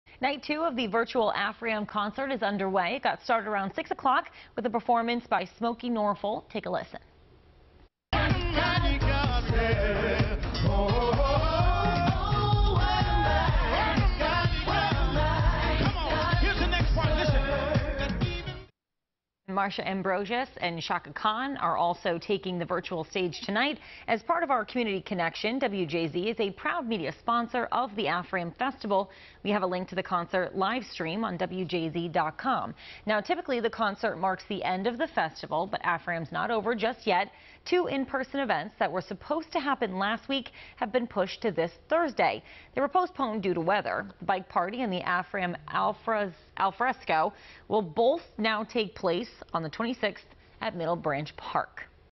Night 2 Of Virtual AFRAM Concert Underway
Stream the performances of Chaka Khan, Smokie Norful and Marsha Ambrosius live on WJZ.com